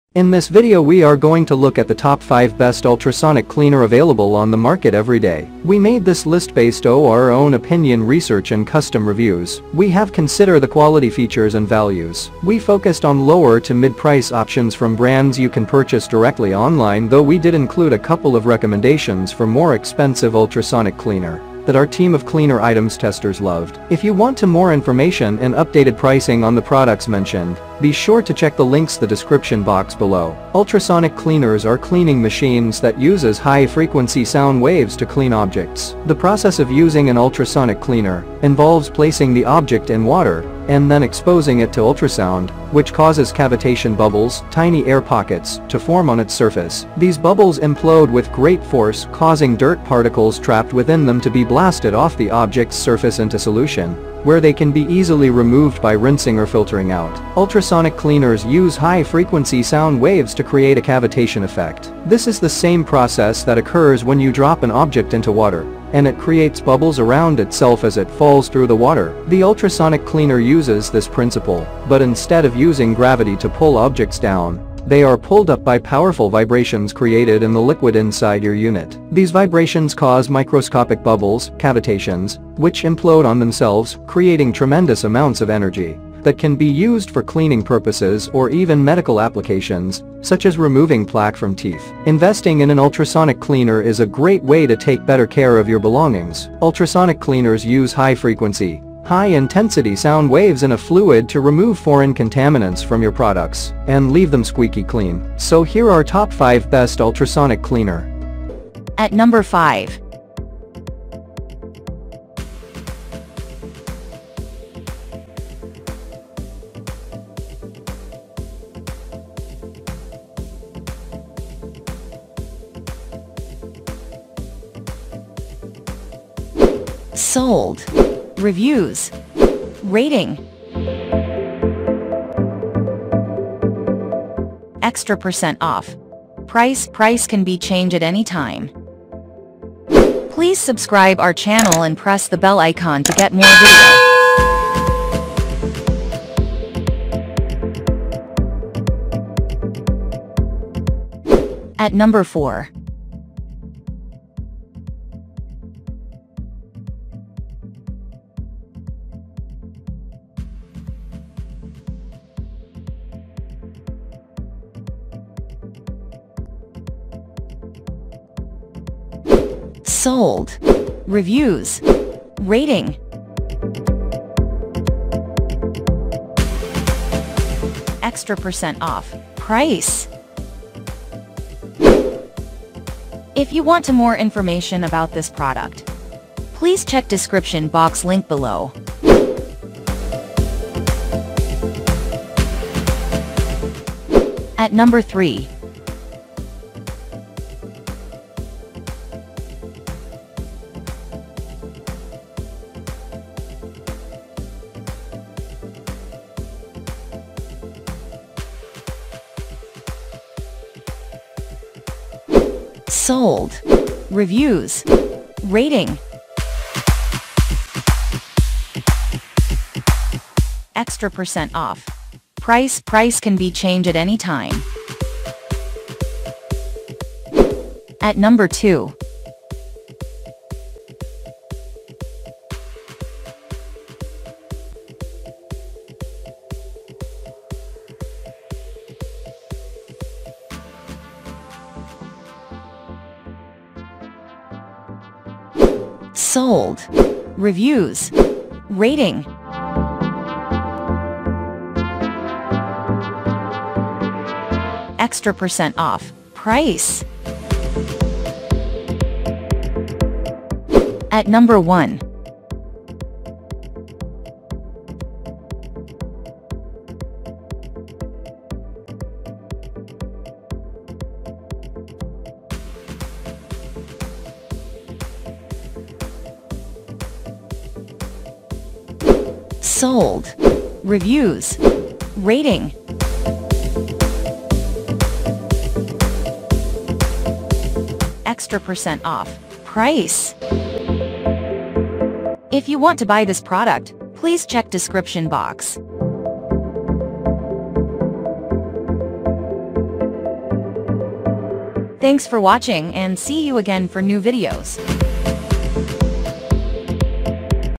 TOP 5 Best Ultrasonic Cleaner Review In 2023
Check The Link for Buy & Details:
1. Digital Ultrasonic Cleaner 3.2L 180W Degas Stainless Bath 40KHz Timer

2.15L Ultrasonic Cleaner Digital Ultrasound Bath Vibration Washer for Home Appliance

3. VEVOR 2/3/6/10/15/22/30L 600W Ultrasonic Machine Ultrasonic cleaner with Heating

4.2L 60W Digital Ultrasonic Cleaner,110V and 220V 40khz price includes cleaning basket

5.Lab Ultrasonic Cleaner 4.5L 180W 40KHz Multi-Feature Digital Degas Semiwave